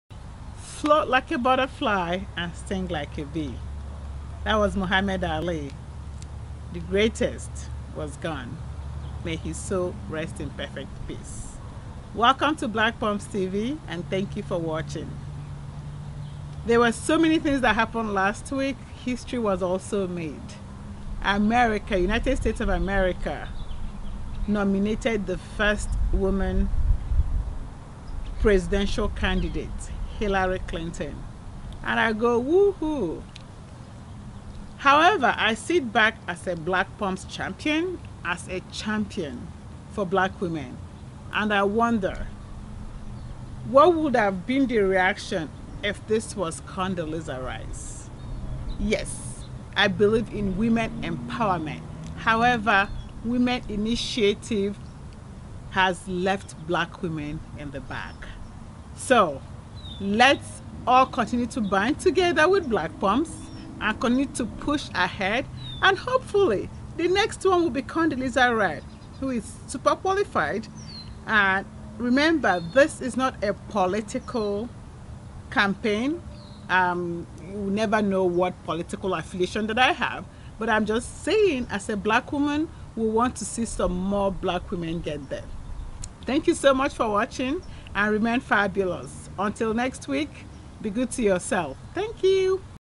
History was made| First woman presidential candidate of a major party was nominated.
Congrats Hilary Clinton! Women Initiative is well and alive. We hope to see more black women executives and party leaders in America.